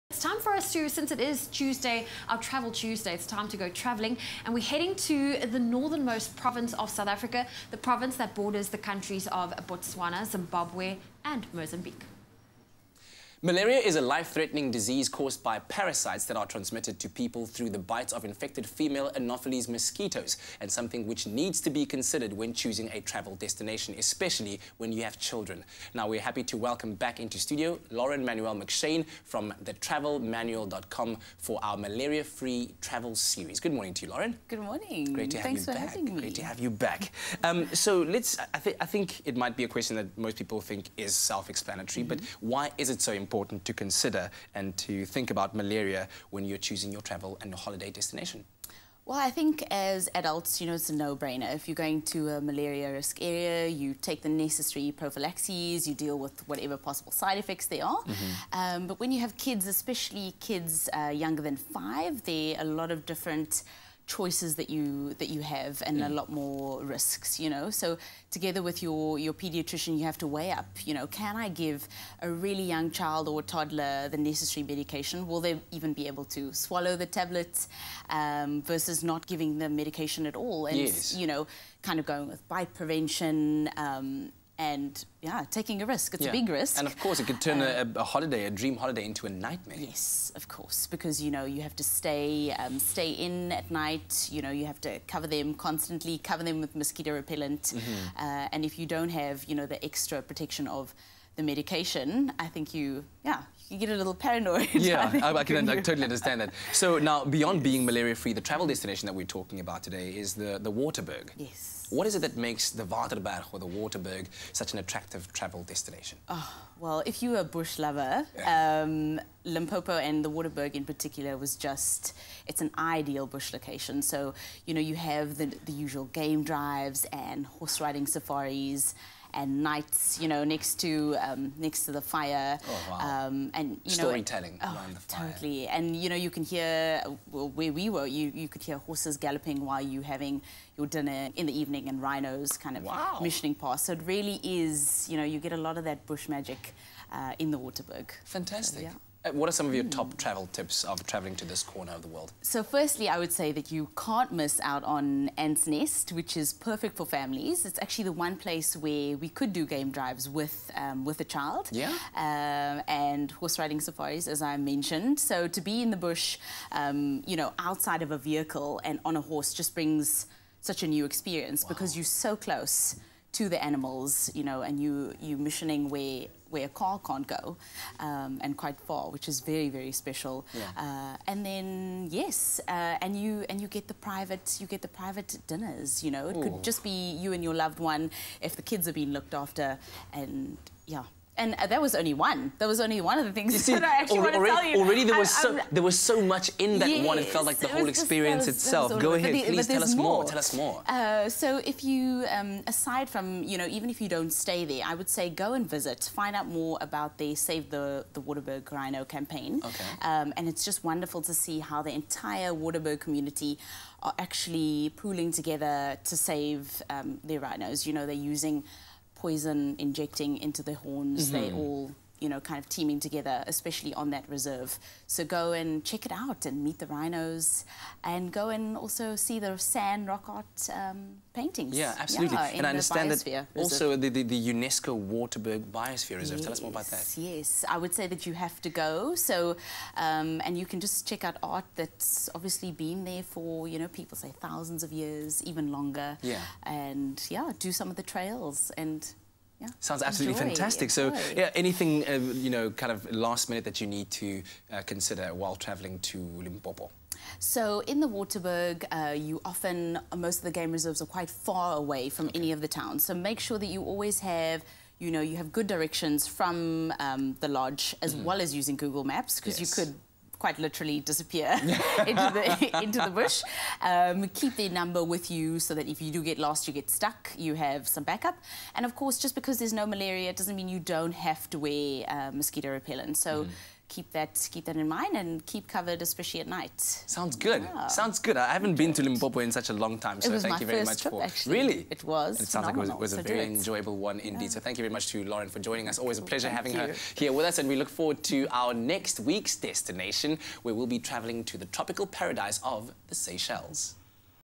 Malaria Free Travel Series – Waterberg / Limpopo
We explore the Waterberg as a malaria free travel destination with Lauren from thetravelmanuel.com